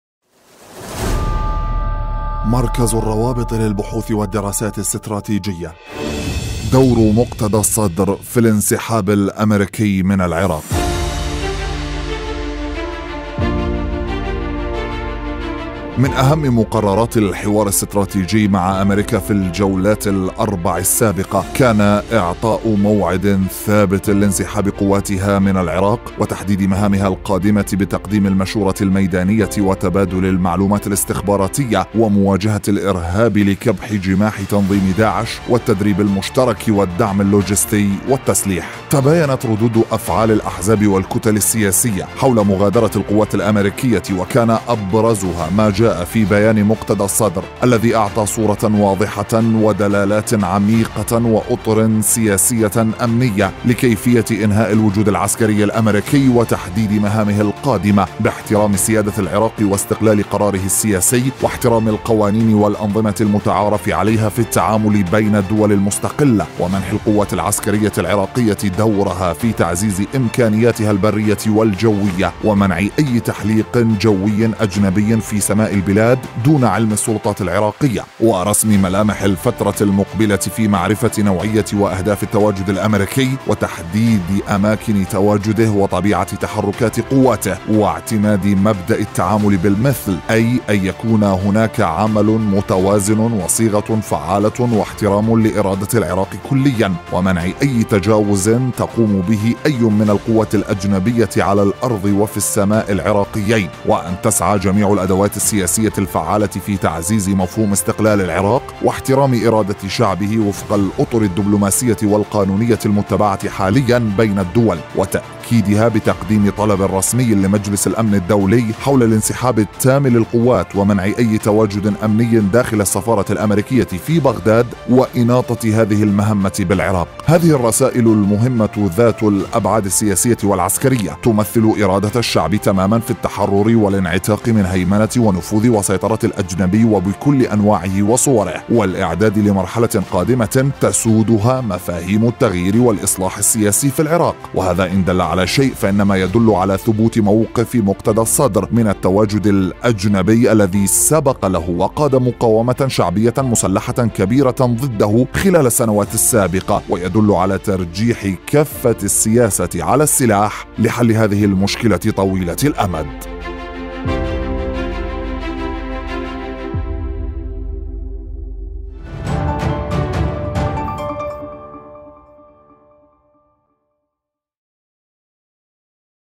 مقتدى الصدر يفعلها.. أمريكا خارج العراق | تقرير
كيف انهى مقتدى الصدر الوجود الامريكي في العراق